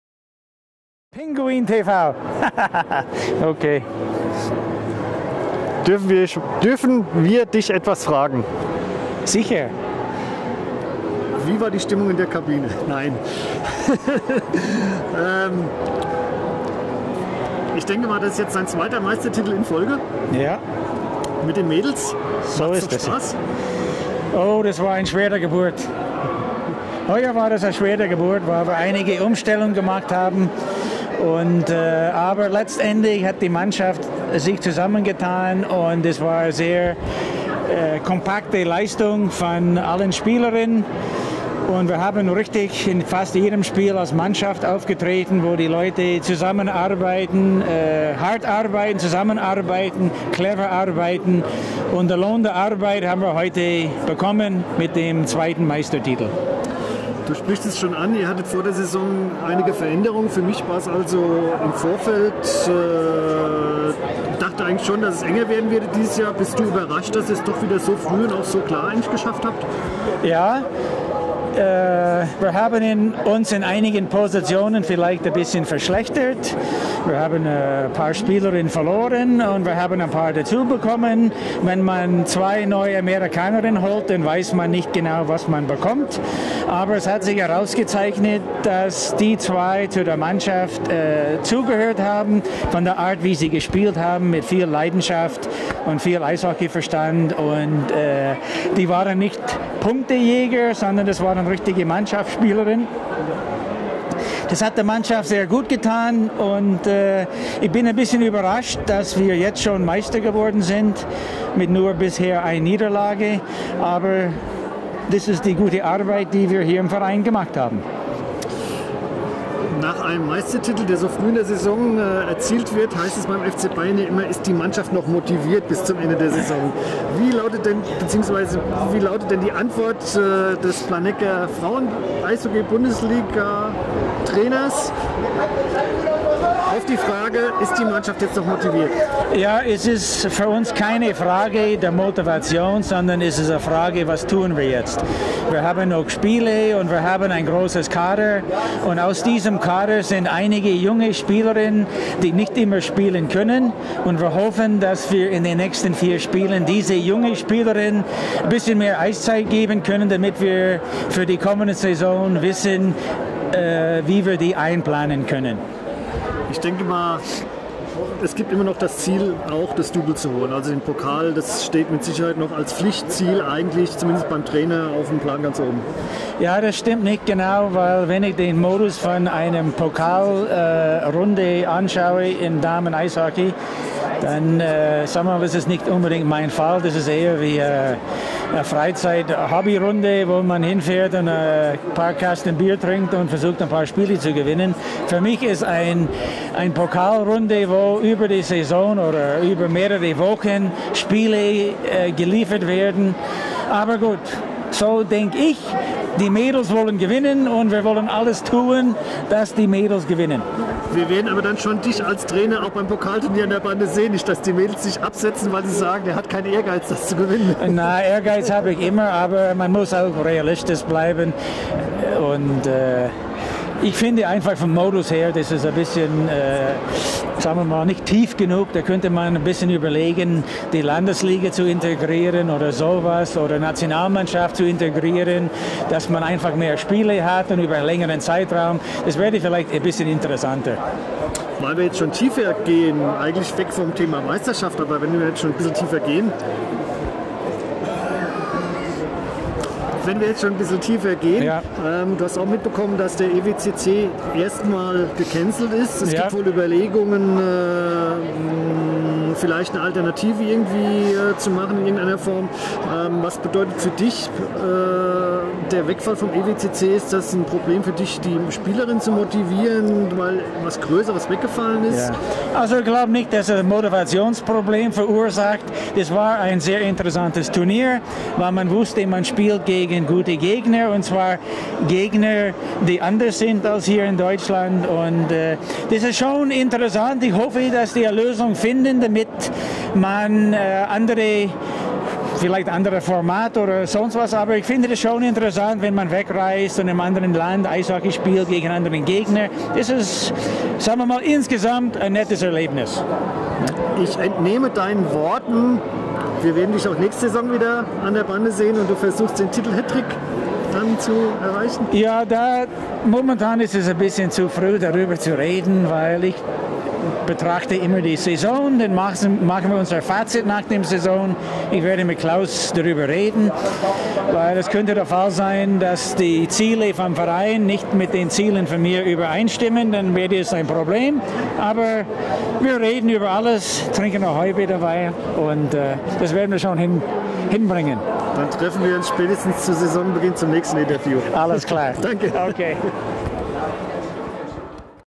Brian Ashton (ESC Planegg - 1.2.2015)
Nach der 6. Deutschen Meisterschaft des ESC Planegg
Rainer Gmach im Gespräch mit Trainer Brian Ashton